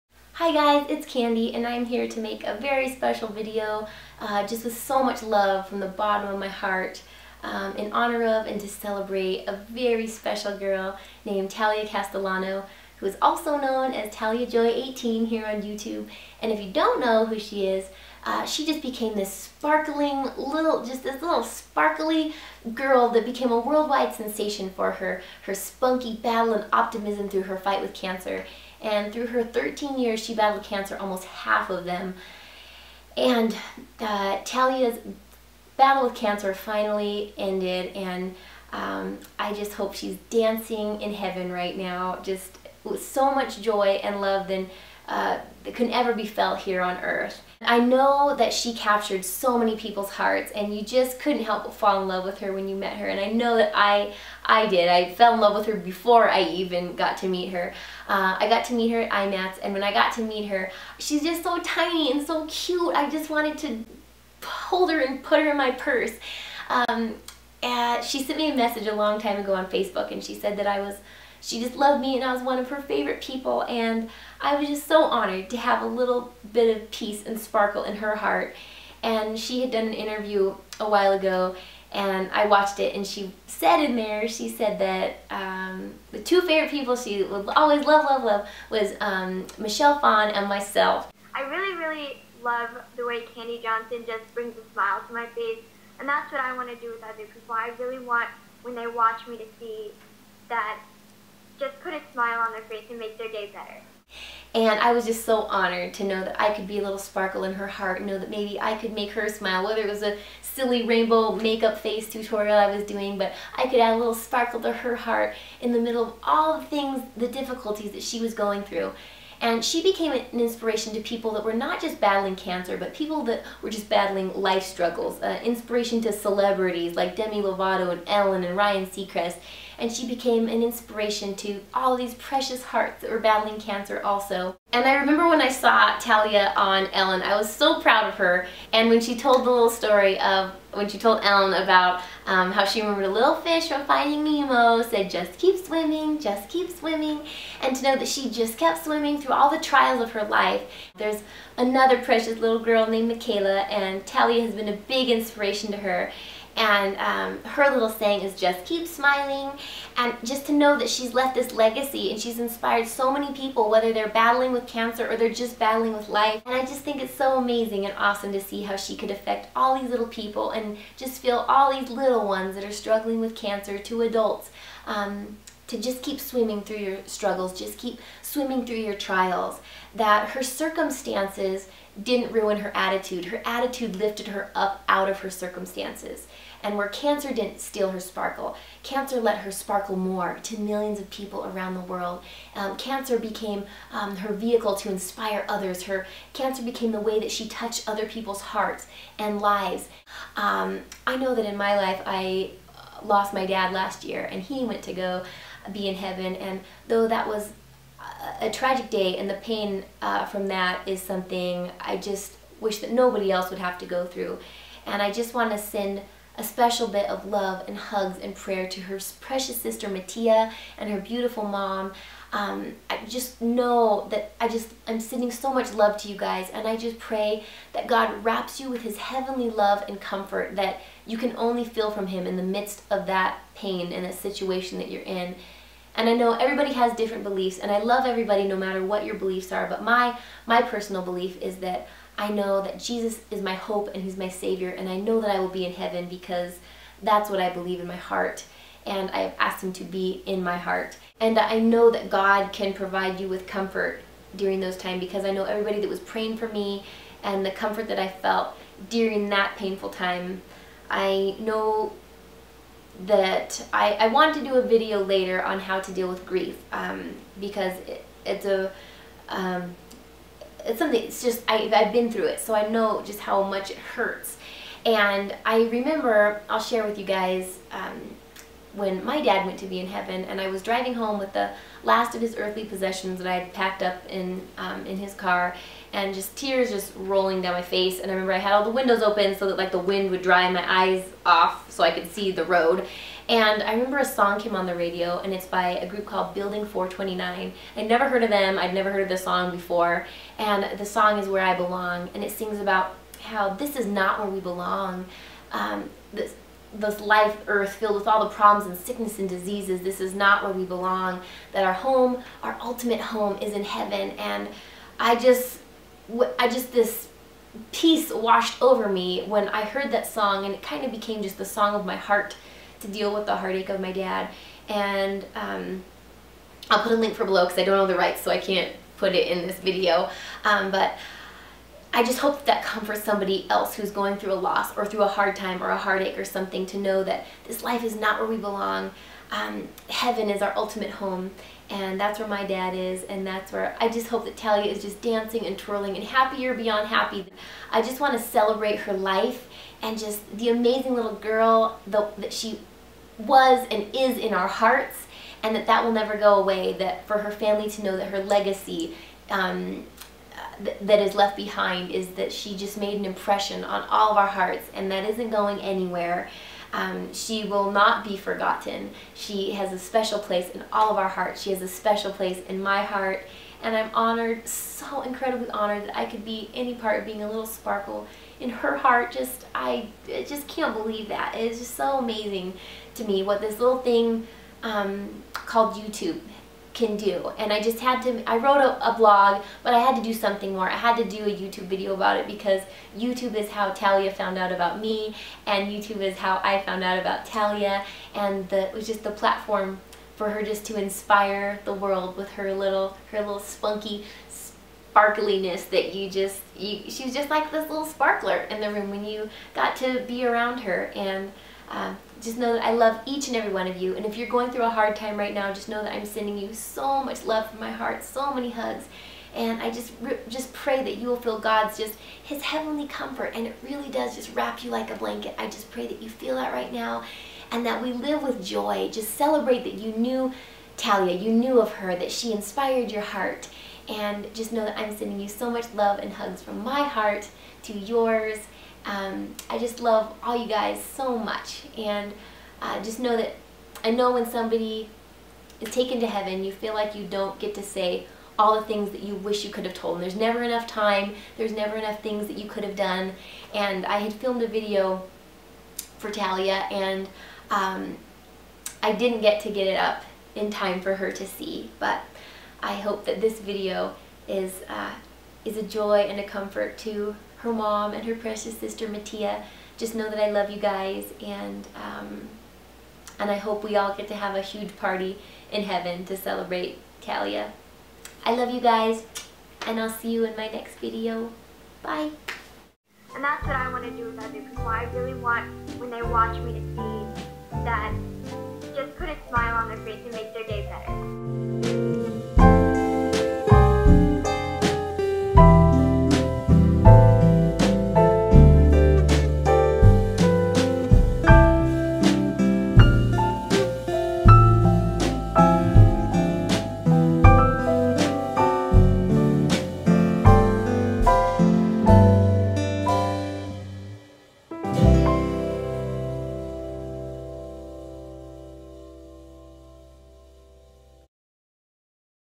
In Honor Of Talia Joy Castellano | Kandee Johnson
With love and gratitude, thank you Mattia and Desiree for sharing, precious Talia with us all. This is to honor Talia Joy Castellano, who will always be a sparkle in our hearts.

If you want to read the blog post I wrote on tuesday, you can read it here:

Talia would want us all to "just keep swimming"...

Talia sparkled on Earth and now she is sparkling even more in Heaven. My heart and prayers go out to anyone who's lost a loved one, or has a loved one battling cancer...